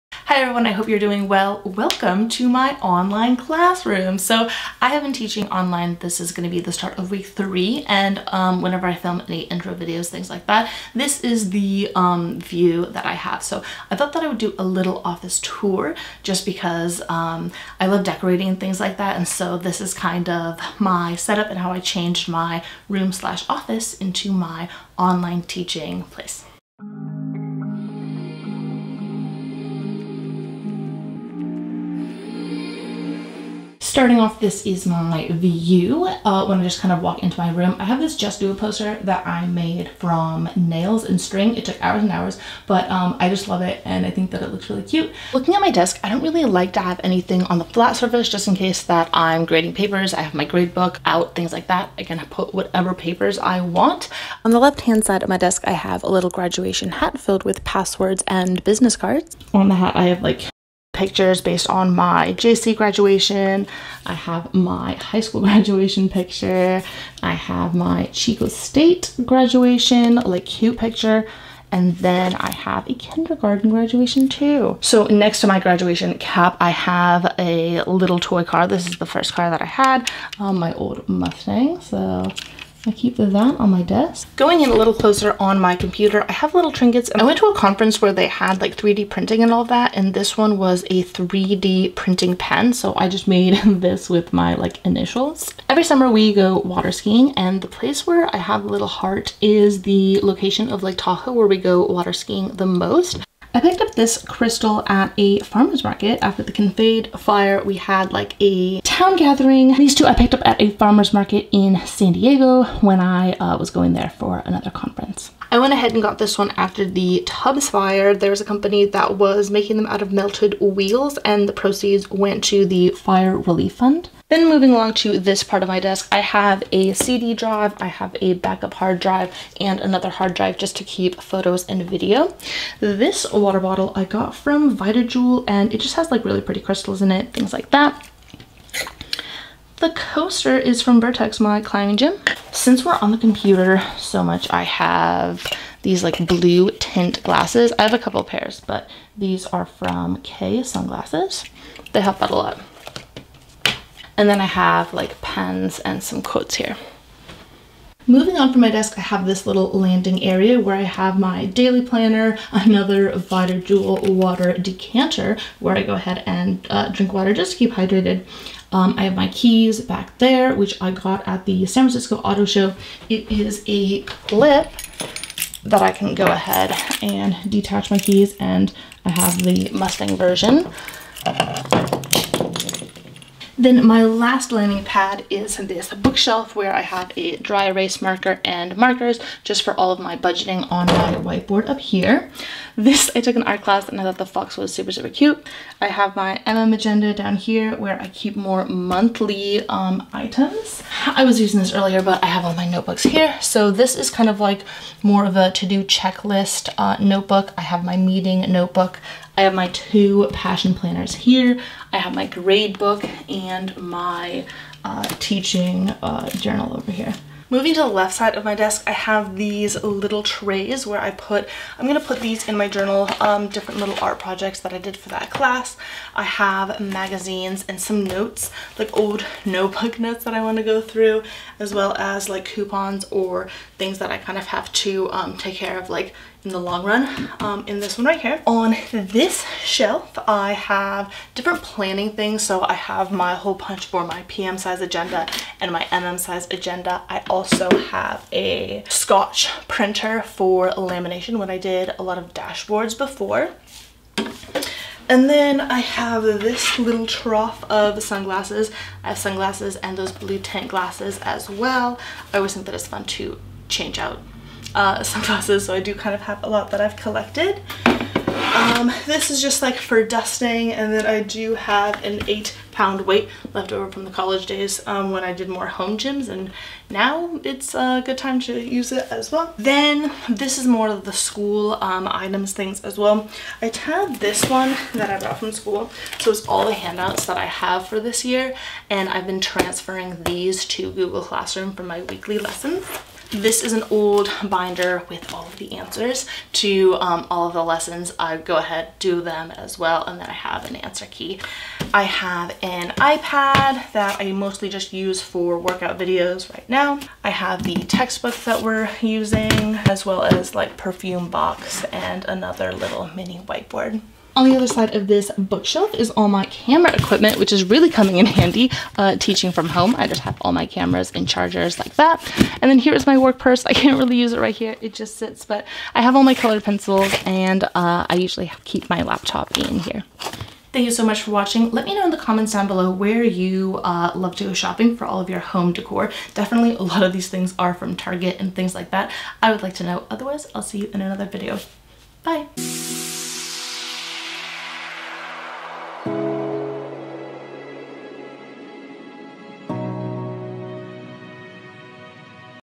Home Office | Work From Home Tour
As I high school science teacher I have transitioned from teaching in my classroom to filming lessons in my home office. Here's a tour of my home office end how I teach from home.